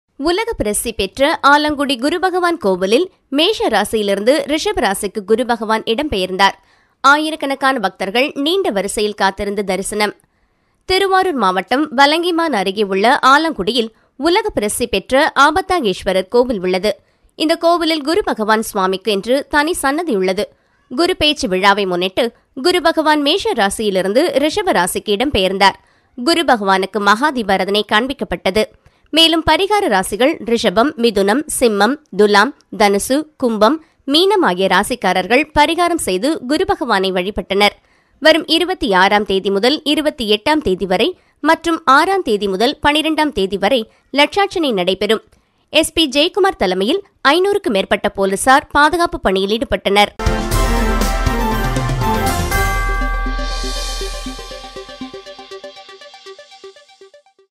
குரு பகவான் கோவிலில் மேஷ ராசியிலிருந்து ரிஷப ராசிக்கு குரு பகவான் இடம் பெயர்ந்தார் | Guru Bhagavan
மக்களவைத் தேர்தல் 2024